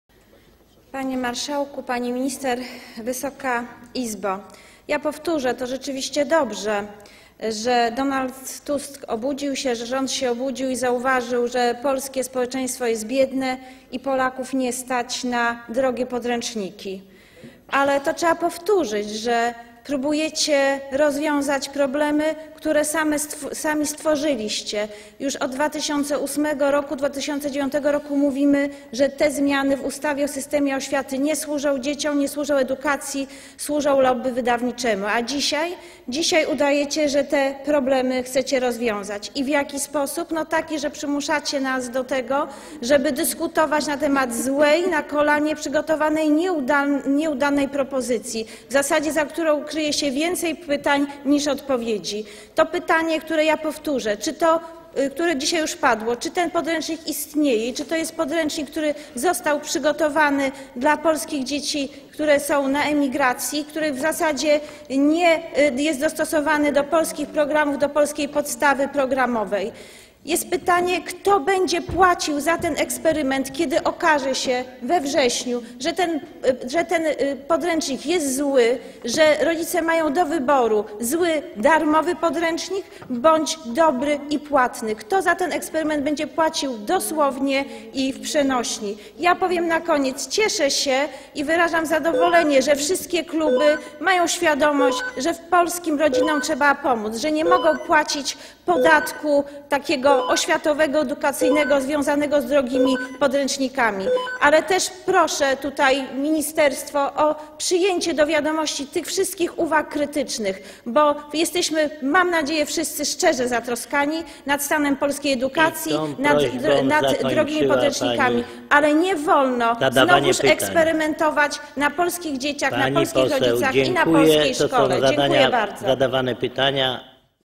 Marzena Machałek - wystąpienie z 19 lutego 2014 r.
Popierasz? Udostępnij! Marzena Machałek - wystąpienie z 19 lutego 2014 r.

Sprawozdanie Komisji o pilnym rządowym projekcie ustawy o zmianie ustawy o systemie oświaty